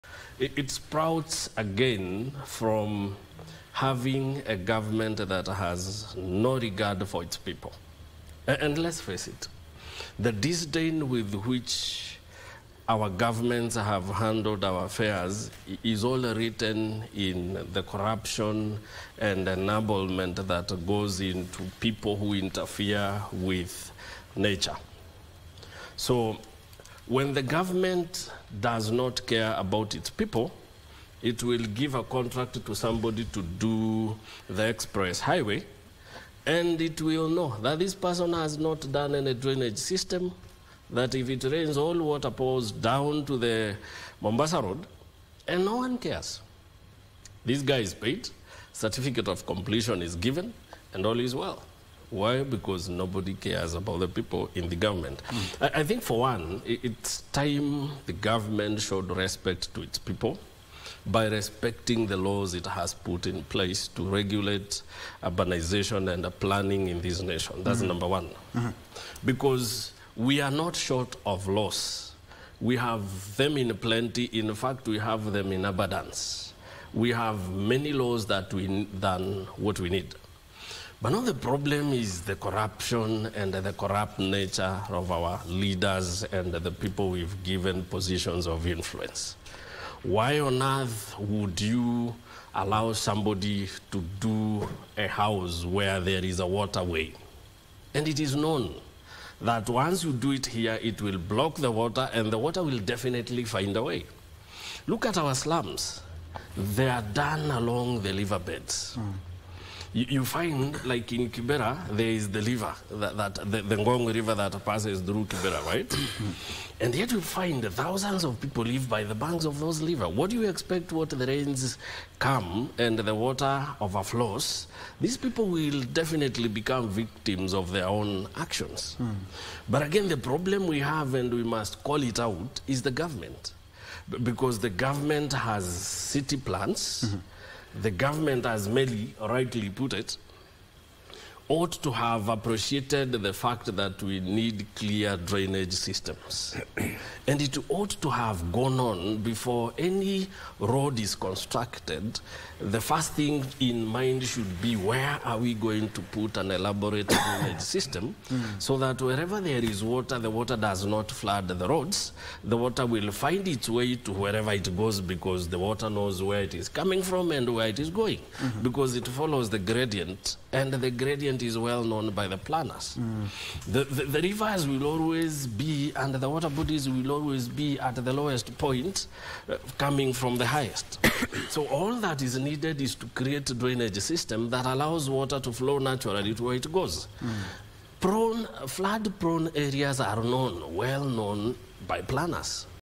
Kimani Mwangi: Gov't should respect laws it has put in place to regulate urbanization and planning
Perennial Floods
Kimani Mwangi - Governance Expert: It's time Gov't showed consideration to its people by respecting the laws it has put in place to regulate urbanization and planning in this nation.